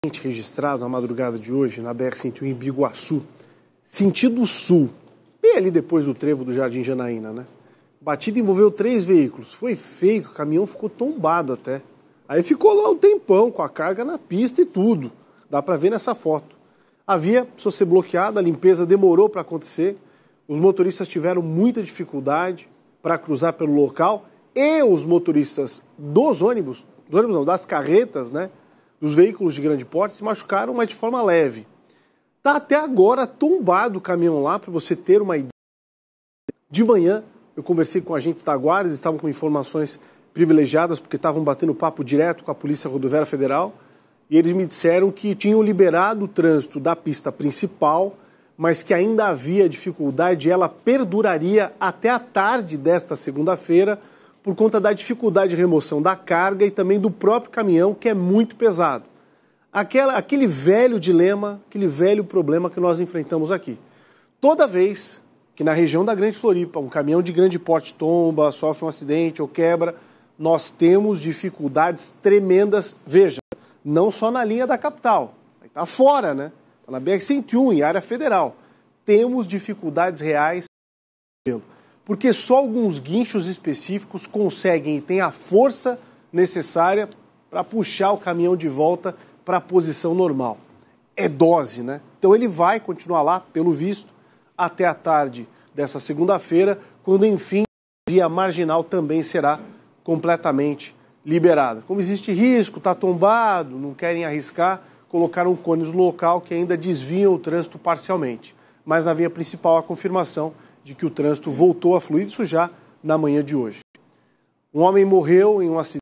Três veículos se envolvem em acidente na BR-101 em Biguaçu
No programa dessa segunda-feira (24) você acompanha todos os detalhes desse acidente.